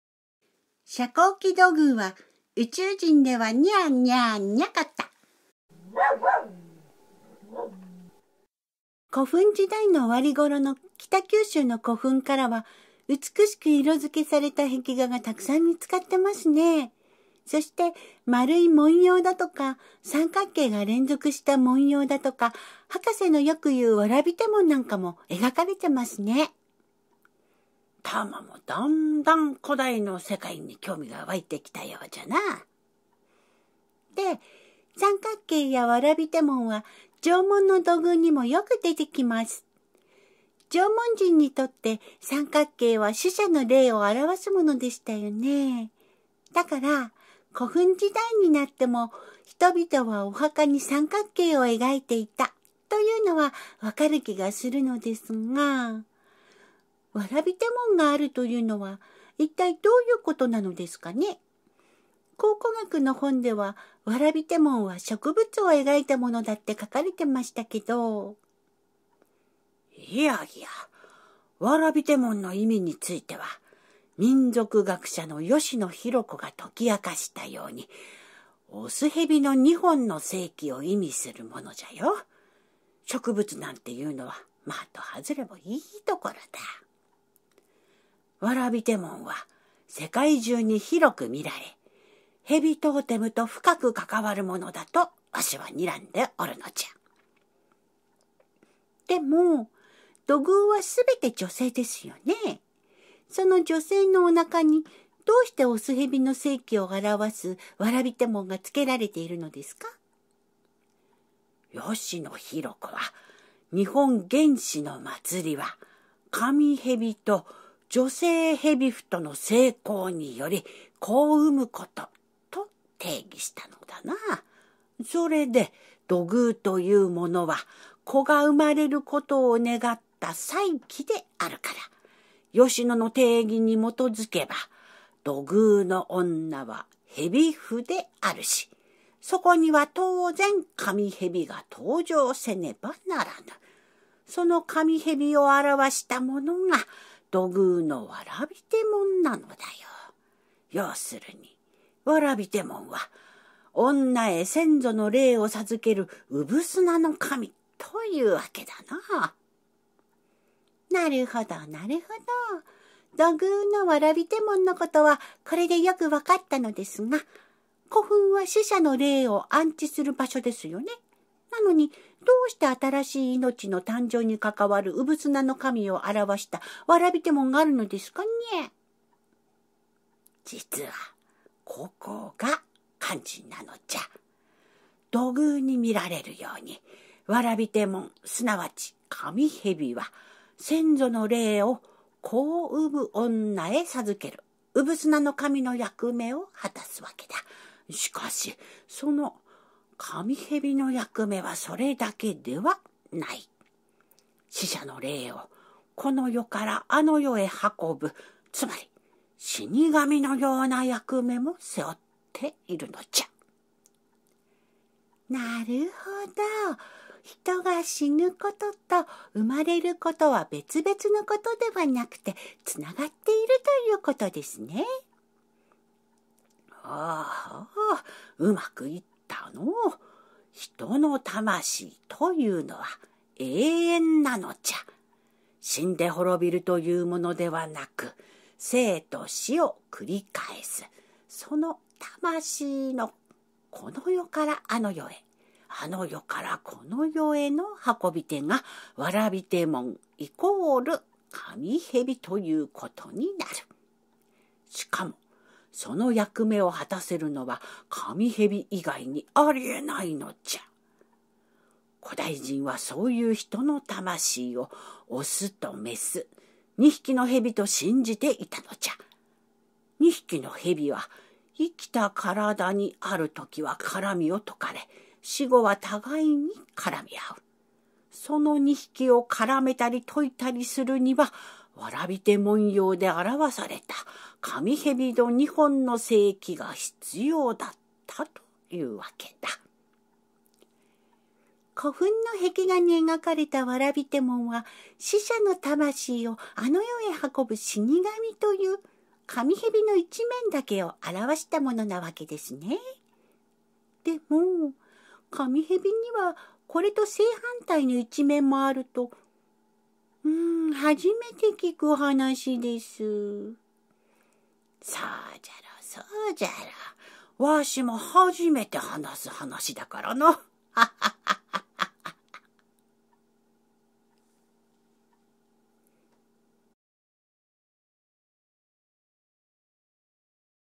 遮光器土偶は、宇宙人ではニャーニャーニャかった16 古墳編 通し版
遮光器土偶が宇宙人でないことは、この本を読めばよくわかる。朗読：ミコ・ツチヤ（亭主に内緒）
佐藤国男著『妄想か、大発見か…　亀ヶ岡土器には甲骨文字が刻まれていた』